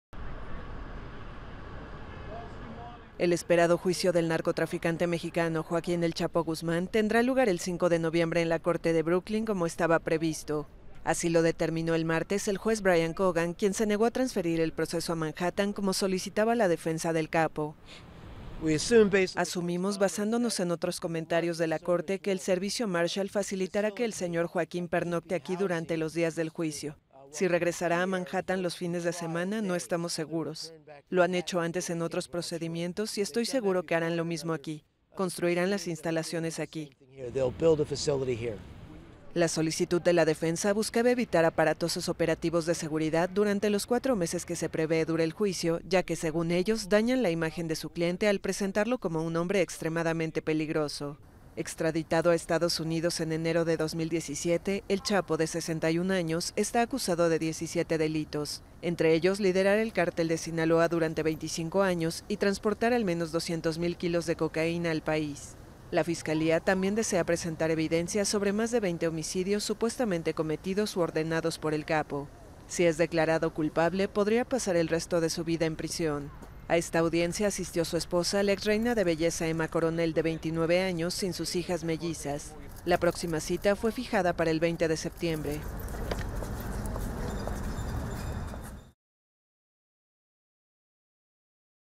Juez de EEUU rechaza transferir juicio de El Chapo.
El aguardado juicio del mexicano "El Chapo" Guzmán, uno de los mayores jefes del narcotráfico del mundo, tendrá lugar a partir del 5 de noviembre en la corte de Brooklyn como estaba previsto, decidió el martes el juez Brian Cogan, al negar transferir el proceso.